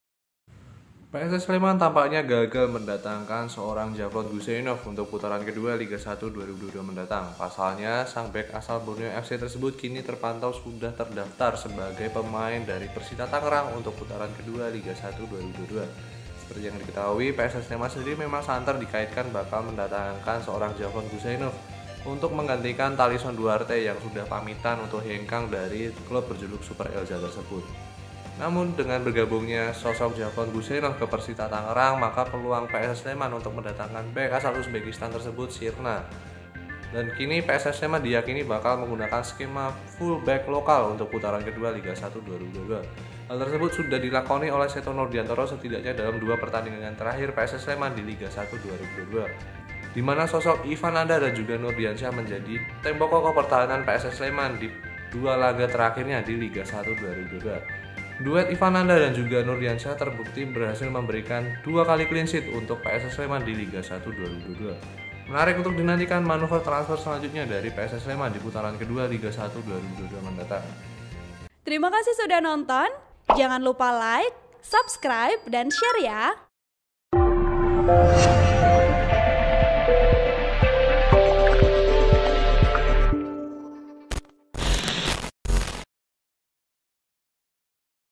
PSS Sleman Gagal Rekrut Javlon Guseynov dan Pilih Skema Full Bek Lokal? Berikut Indikasi Kuatnya
TRIBUN-VIDEO.COM - PSS Sleman diyakini akan memakai skema bek tengah penuh talenta lokal seusai gagal menggaet sosok Javlon Guseynov untuk putaran kedua Liga 1 2022.

Dilansir TribunWow.com, PSS Sleman sendiri memang sedang santer dikaitkan dengan beberapa bek asing untuk mengganti sosok Tallysson Duarte yang telah pamitan dari klub berjuluk Super Elja tersebut.

Tallysson Duarte menjadi pemain asing ketiga PSS Sleman yang hengkang dari Super Elja di putaran kedua Liga 1 2022 menyusul sosok Ze Valente dan Mychell Chagas.

PSS Sleman hanya menyisakan Jihad Ayoub seorang untuk menyongsong putaran kedua Liga 1 2022 dan mendatangkan nama-nama seperti Yevhen Bokhashvili dan Jonathan Cantillana.

Demi menggantikan peran Tallysson Duarte, PSS Sleman sempat dikaitkan dengan bek tangguh Borneo FC, Javlon Guseynov.

Dilansir TribunWow.com, posisi Javlon Guseynov yang tergeser di Borneo FC diyakini menjadi alasan kuat PSS Sleman ingin menggaet bek asal Uzbekistan tersebut.

Sebelumnya, Borneo FC resmi mendatangkan sosok Julio Cesar, bek asal Brasil pada putaran kedua Liga 1 2022 dan membuat posisi dari Javlon Guseynov tersingkir dari skuad utama Pesut Etam.

Tersingkirnya Javlon Guseynov dari skuad Borneo FC di putaran kedua Liga 1 2022 mencoba dimanfaatkan oleh PSS Sleman dan PSIS Semarang.

Namun, PSIS Semarang yang sudah memastikan bahwa akan memakai pemain bertahan lokal membuat PSS Sleman selangkah lebih maju dari Laskar Mahesa Jenar untuk menggaet Javlon Guseynov.

Kini, PSS Sleman tampaknya harus gigit jari karena telah gagal mendatangkan sosok Javlon Guseynov di putaran kedua Liga 1 2022

Artikel ini telah tayang di TribunWow.com dengan judul PSS Sleman Gagal Rekrut Javlon Guseynov dan Pilih Skema Full Bek Lokal? Berikut Indikasi Kuatnya,

Penulis: Aulia Majid
Editor: Tiffany Marantika Dewi
VP: Firdausy Shabrina